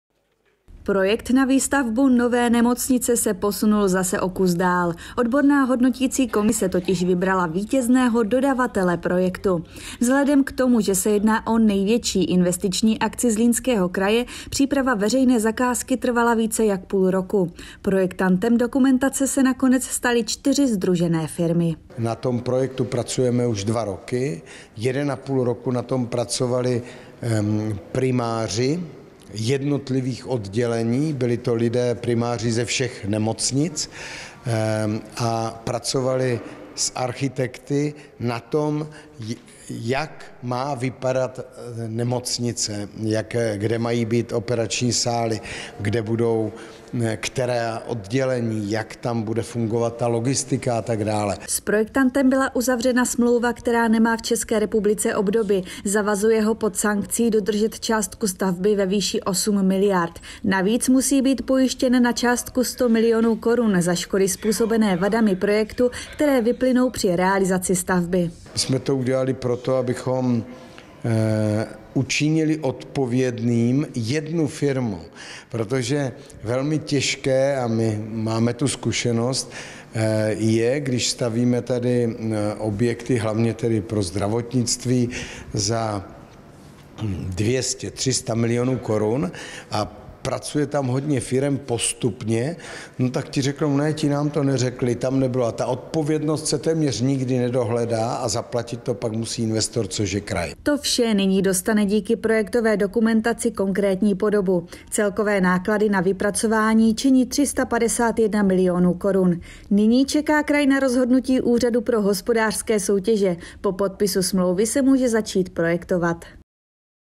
Kraj vybral projektanta nové nemocnice
Rada Zlínského kraje schválila dodavatele projektové dokumentace na Novou krajskou nemocnici ve Zlíně.